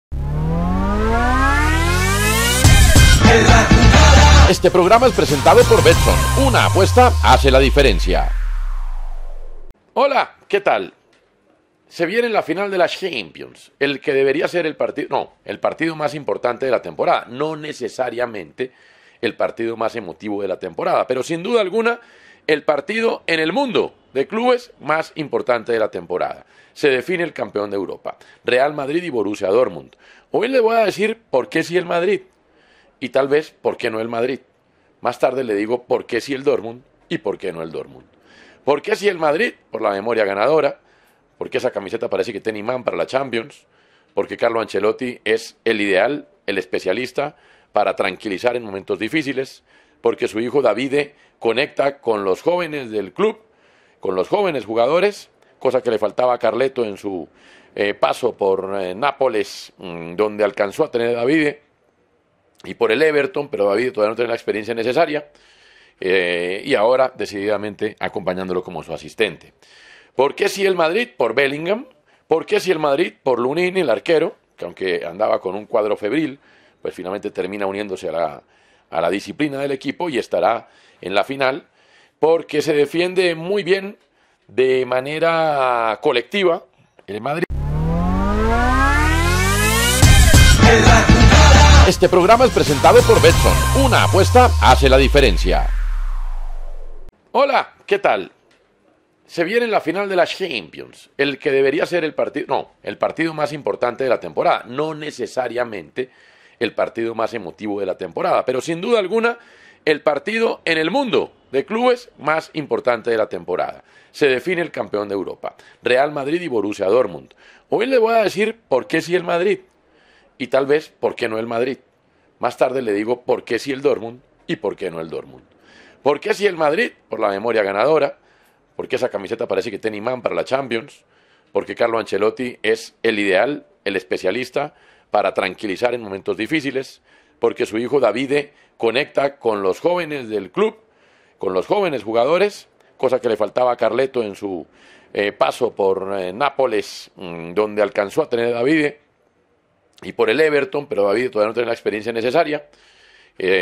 Champions League: se vivirá el partido más importante de la temporada
Real Madrid y Borussia Dortmund se enfrentarán en la final de la Champions League. Un partido que detiene el mundo y pone nerviosos a los aficionados.
@betsson_colombia, una apuesta hace la diferencia